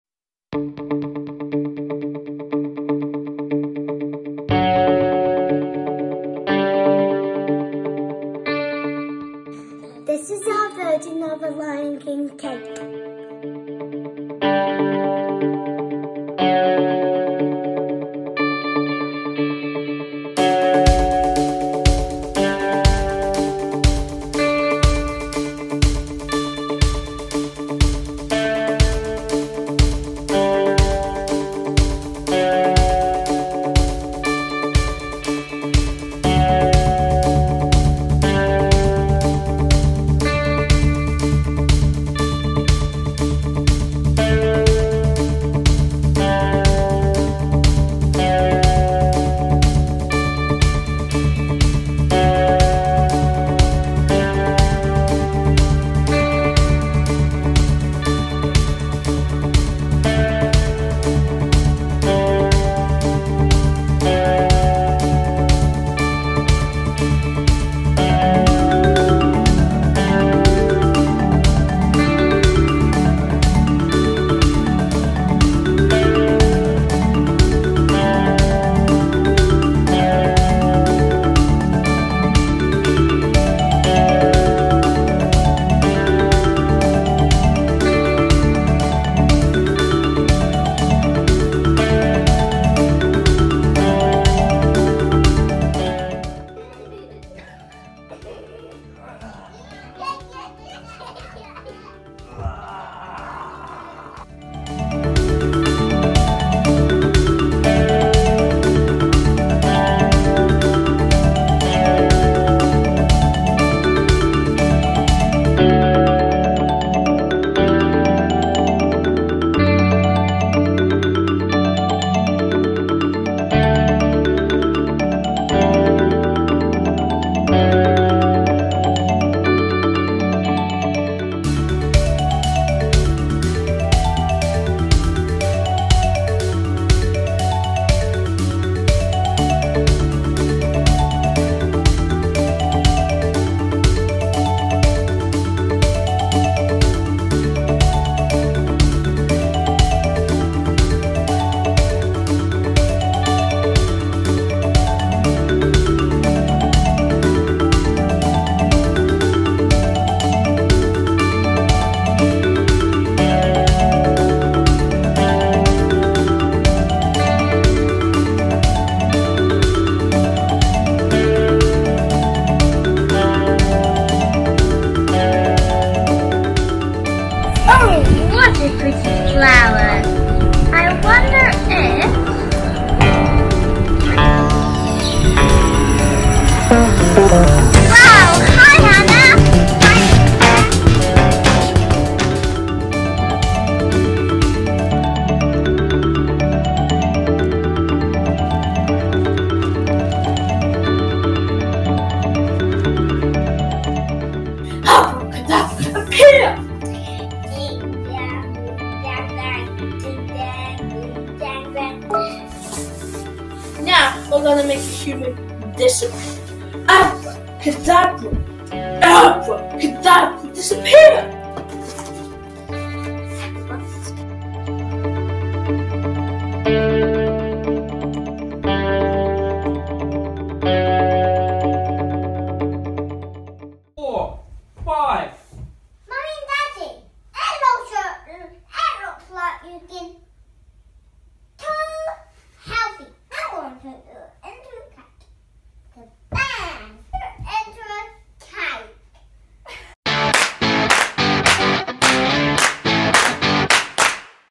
Home Challenges#2
This week's challenges were to bake a cake, get physical, share your lockdown art or do a magic trick. Some very creative ākonga at Te kura Pokapū o Whakatū.